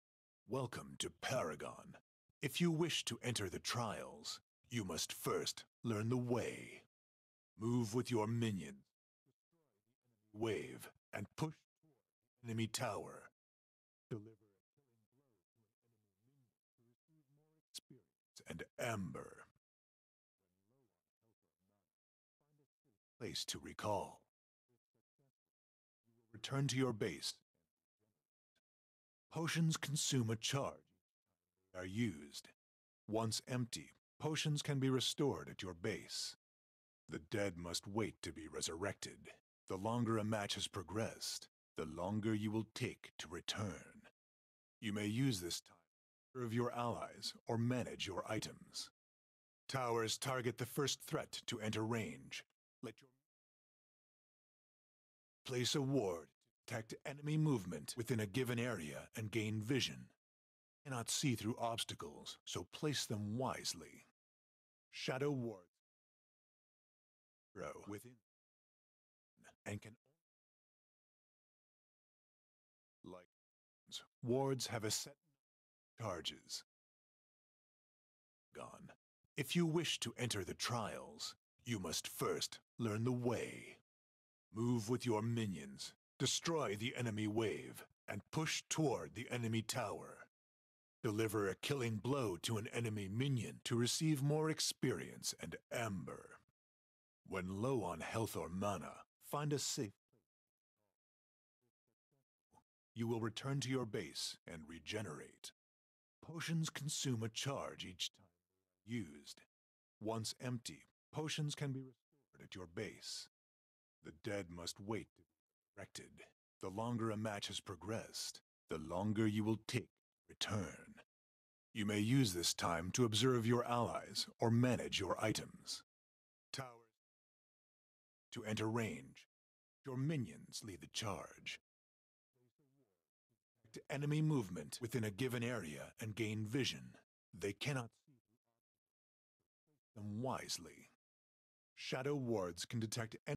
SteamAudio: Dynamic Geometry Demo for UE4.25
As of Steam Audio Beta 17, we support dynamic geometry when modelling all sound propagation effects, including occlusion, transmission, source-dependent reflections, and listener-centric reverb. At this time, this functionality requires the use of the Embree ray tracer.